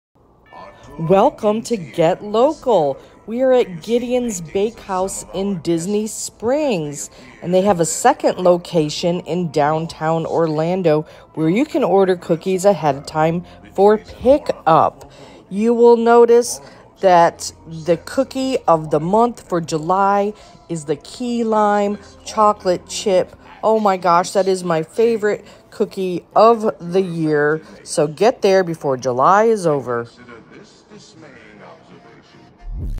Get Local ~ Gideon's Bakehouse in Disney Springs
Get Local! Gideon’s Bakehouse is arguably the best bakery in Orlando. The cookies and cakes are divine. The October cookie of the month is Frankenstein and Bride of Frankenstein. They are also known for their cold brew. The Gideon’s staff highly recommends the peanut butter cold brew.